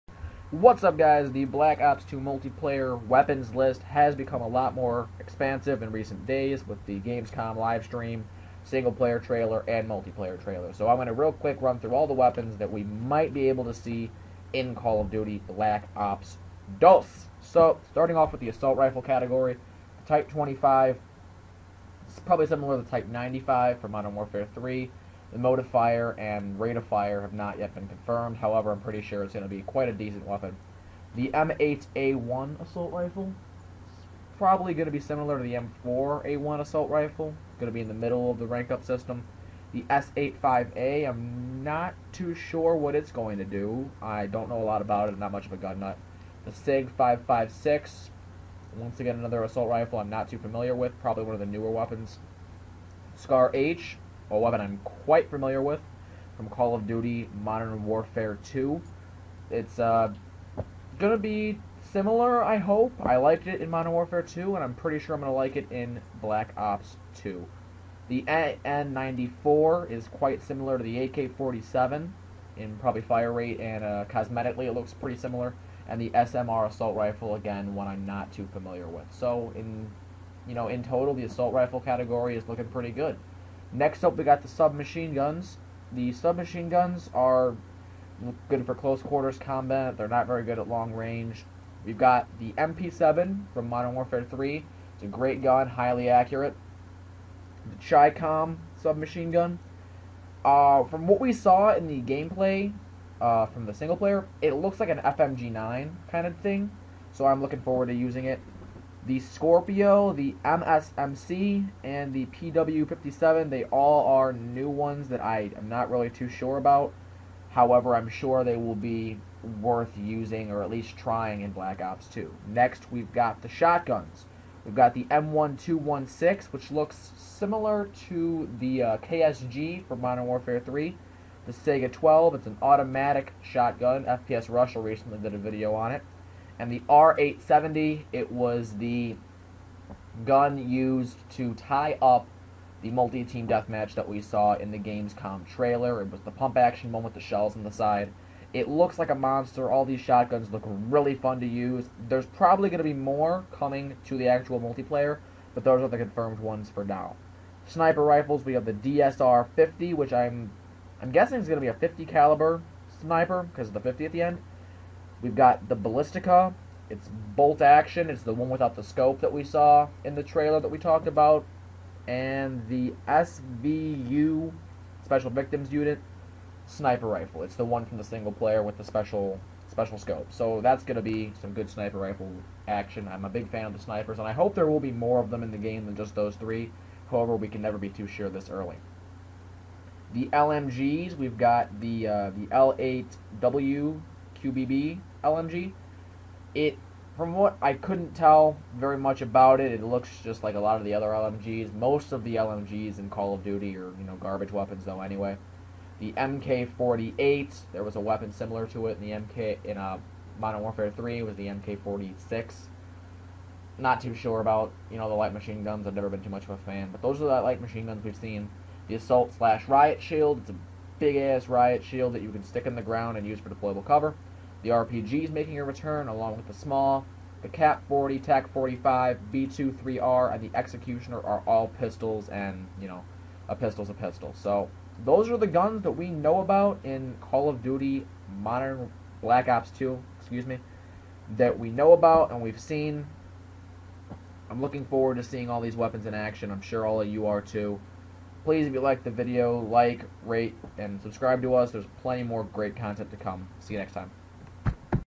Black ops 2 weapons list Confirmed
Jimmy goes over a comprehensive list of weapons we can hope to see in black ops 2!